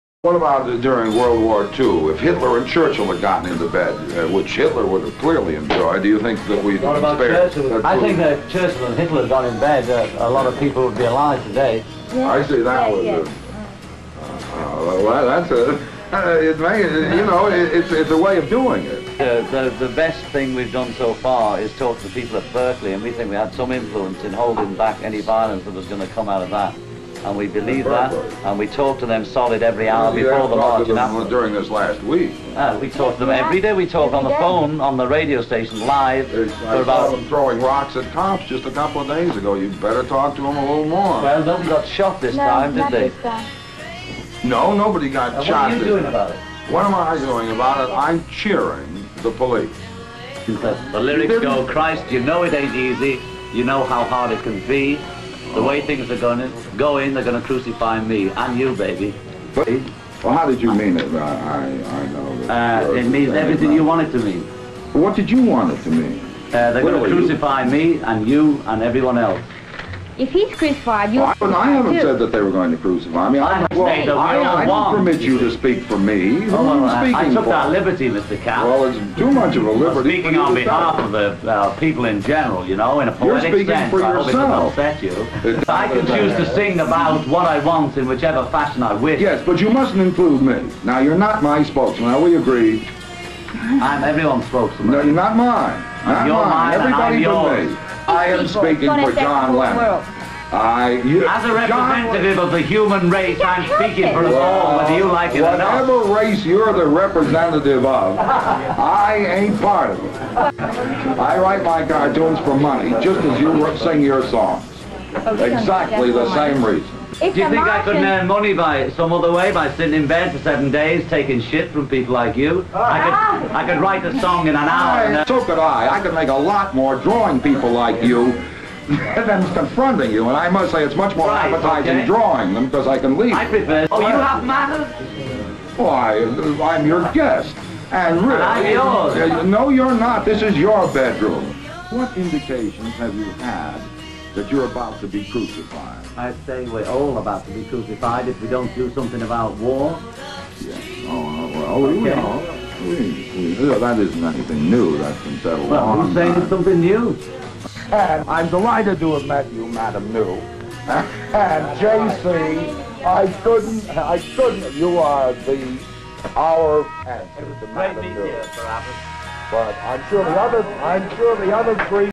John Lennon Interview With Rude Reporter Montreal 1969
yoko ono al capp timothy leary